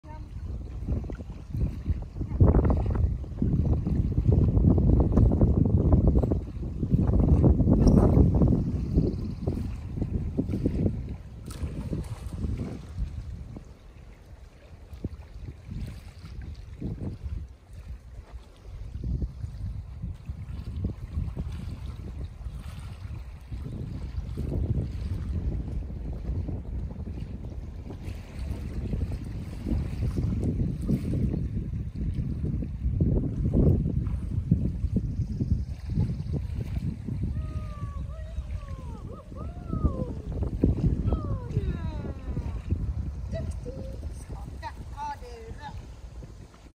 Bojan apporterar i vatten
Bojan, ca 11 mån, tränar för spanielns vattenprov. Då behöver man kunna simma ut och hämta en and (efter skott och kast) och lämna av den. Jag försöker undvika för mycket kast på vatten eftersom det oftast drar upp hundarna onödigt mycket. Istället gör vi linjetag. Bojan har lärt sig att hålla fast och inte skaka sig förrän efter avlämningen, så det gör hon briljant!